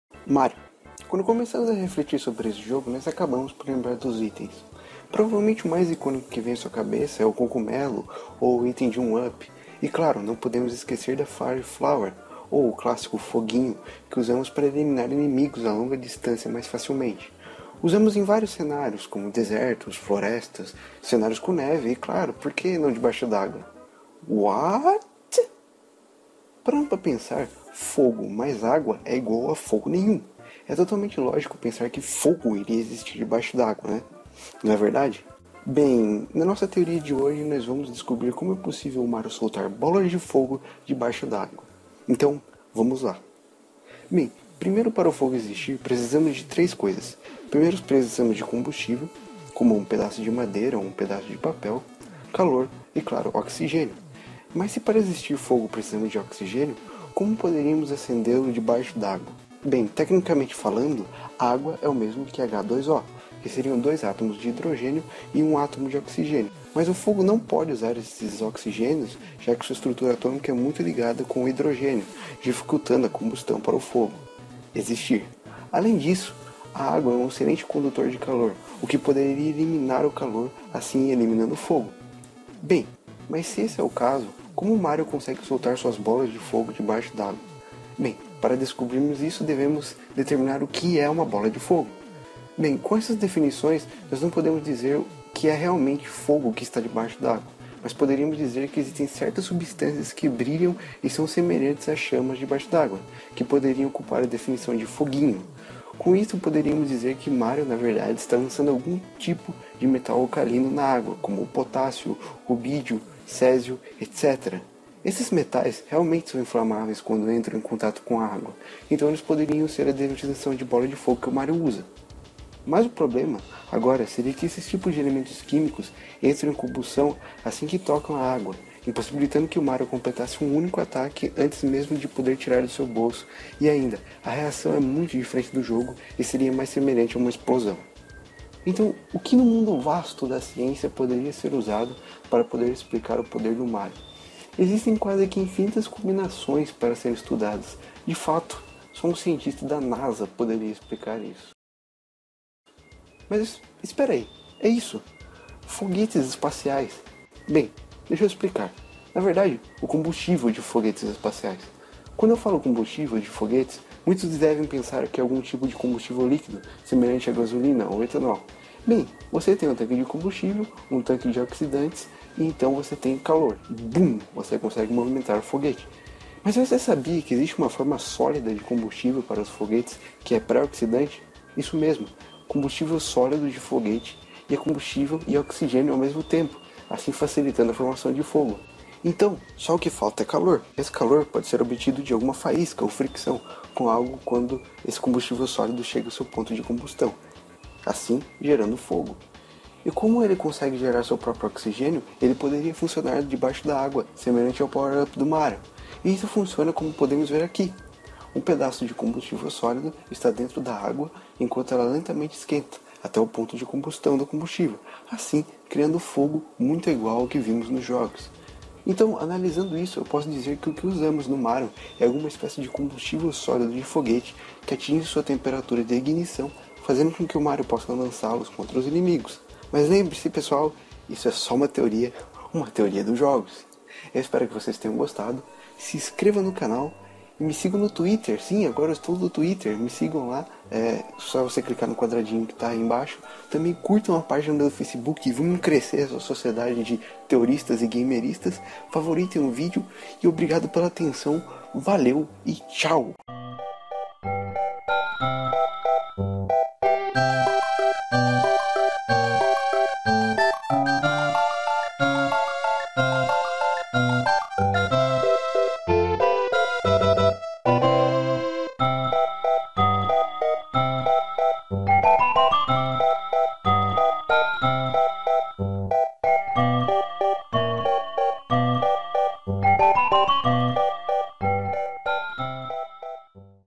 Teoria dos Jogos I Como o Mario Solta Fogo na Água ?
Nesse Vídeo Você Aprende :
- O Que é Metal Alcalino
- Como Funciona um Combustível de Foguete Espacial
- Como o Fogo Funciona